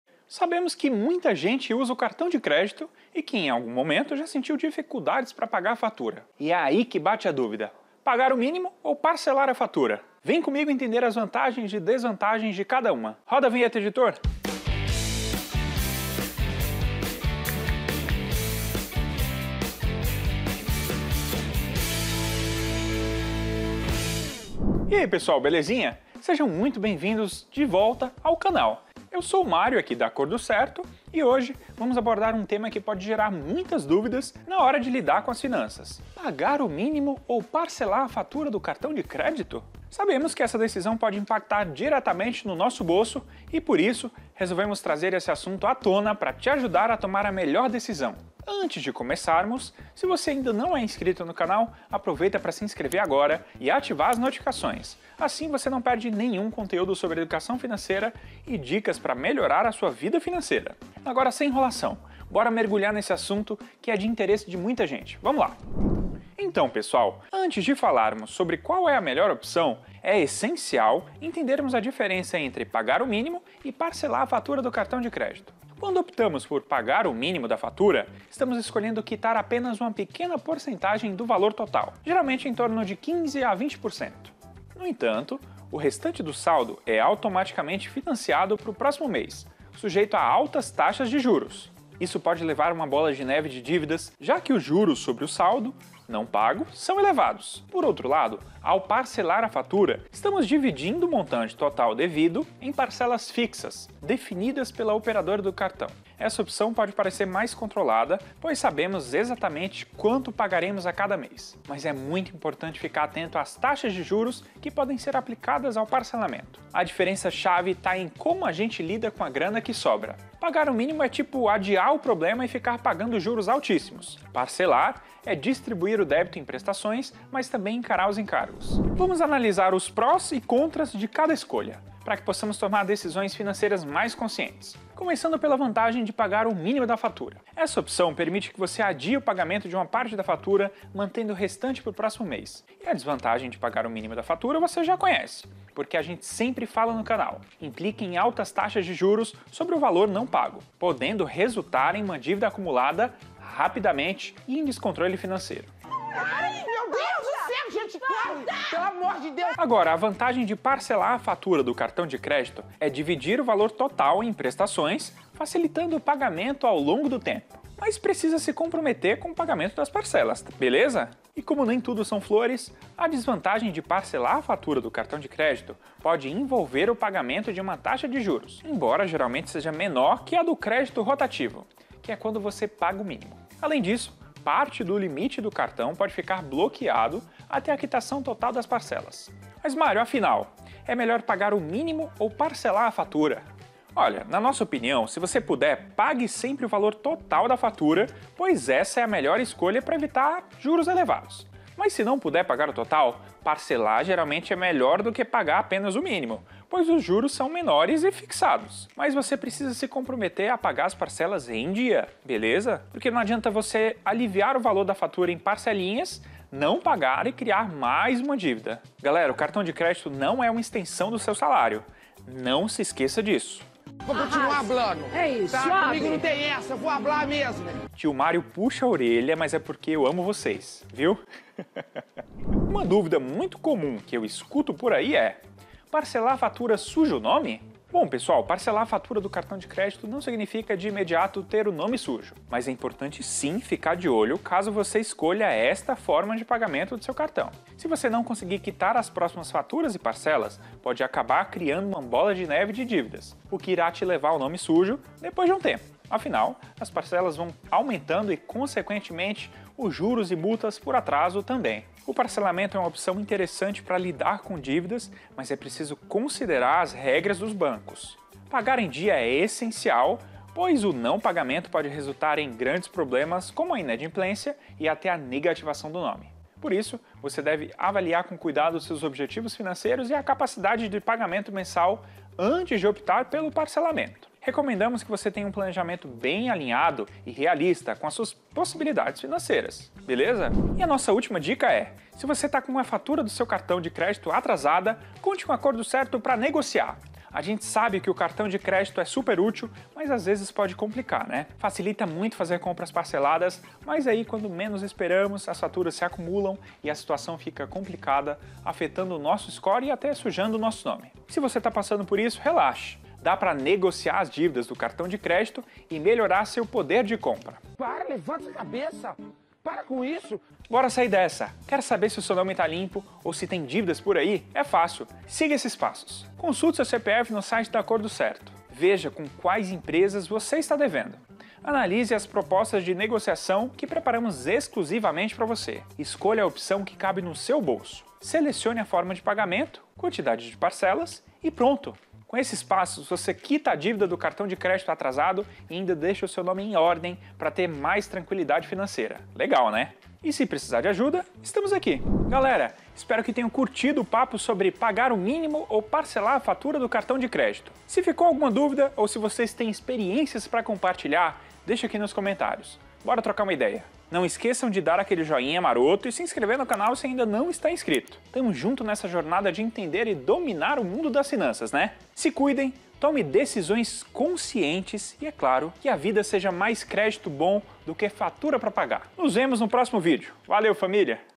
Pagar o Mínimo do Cartão ou Parcelar a Fatura: O Que É Melhor? - Acordo Certo
Desvende o dilema financeiro: pagar o mínimo do cartão ou parcelar a fatura?

Este vídeo explora as nuances das duas opções, revelando qual estratégia é mais vantajosa para o seu bolso. Descubra dicas valiosas, comparativos e conselhos especializados para tomar a melhor decisão em relação às suas finanças. Assista agora e conquiste o controle financeiro que você merece!

Vá direto ao assunto:

00:00 - Introdução
01:06 - Qual a diferença entre pagar o mínimo do cartão e parcelar a fatura?
02:09 - Qual a vantagem de pagar o mínimo da fatura do cartão?
02:23 - Qual a desvantagem de pagar o mínimo da fatura do cartão?
02:40 - Qual a vantagem de parcelar a fatura do cartão de crédito?
02:51 - Qual a desvantagem de parcelar a fatura do cartão de crédito?
03:09 - Pagar o  mínimo ou parcelar a fatura: qual a melhor opção?
03:52 - Parcelar fatura suja o nome?
04:51 -Estou com a fatura do cartão de crédito atrasada, o que fazer?
05:53 - Encerramento

Acesse e consulte seu CPF gratuitamente.

🔗 Pagar Parcelado ou À Vista? Qual a melhor opção? Confira